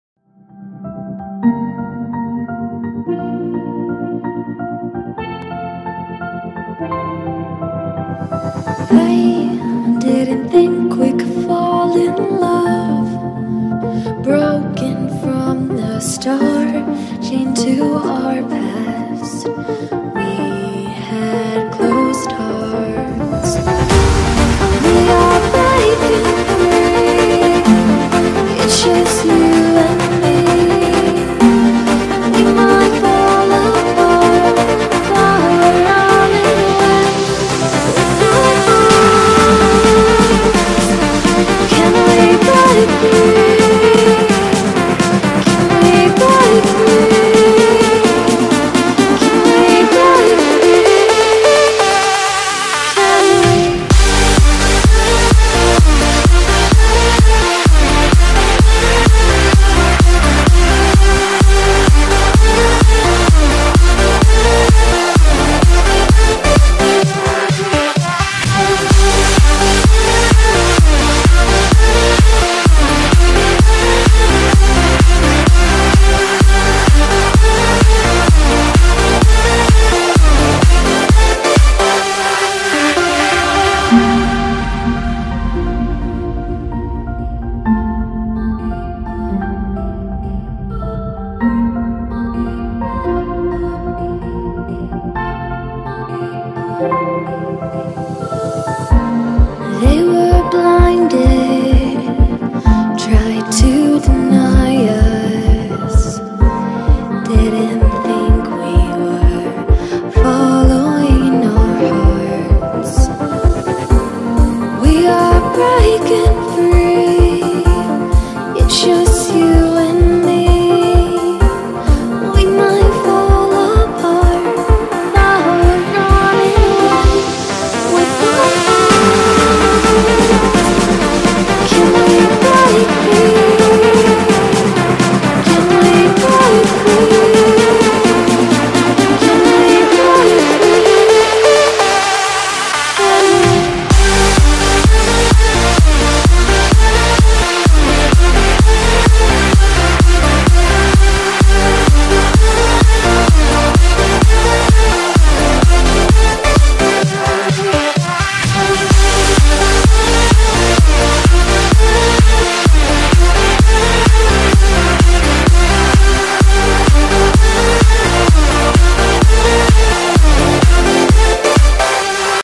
70 let značky Zetor!
Můj mix videí se Zetory. Rozhodl jsem se video udělat z důvodů výročí 70 let značky Zetor!! :)